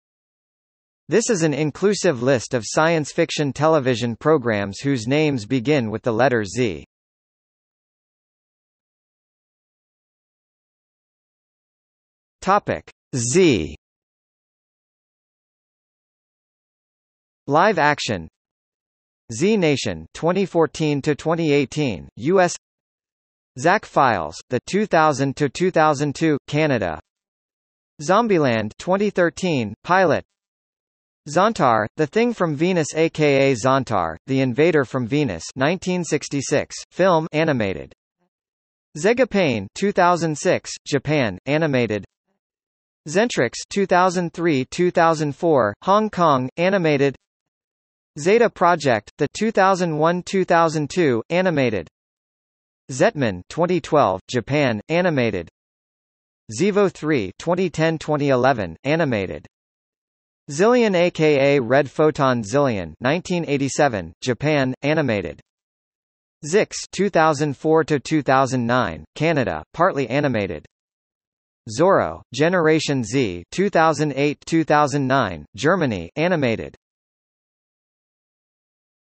List of science fiction television programs, Z | Wikipedia audio article
00:00:08 Z

- Socrates

SUMMARY
This is an inclusive list of science fiction television programs whose names begin with the letter Z.